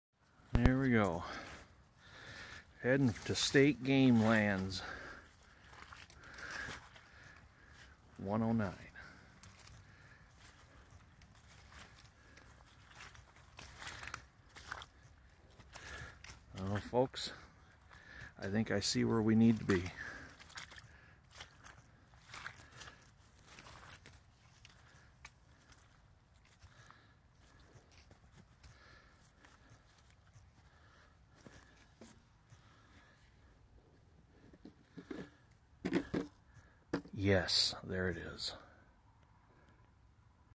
State game lands 109
heading out to state game lands 109 which is Pennsylvania's oldest geocache.